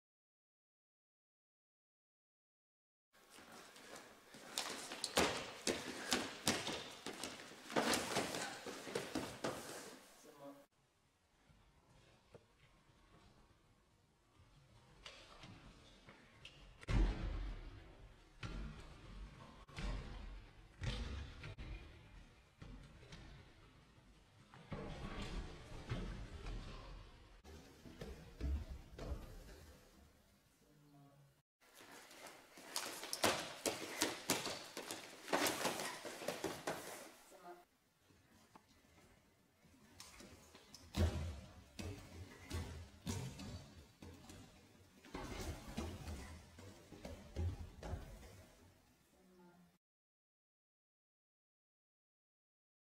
combination body lock and arm spin
Wrestling technique executed by Nenad and Neven Zugaj.
Olympian, World and European medalist.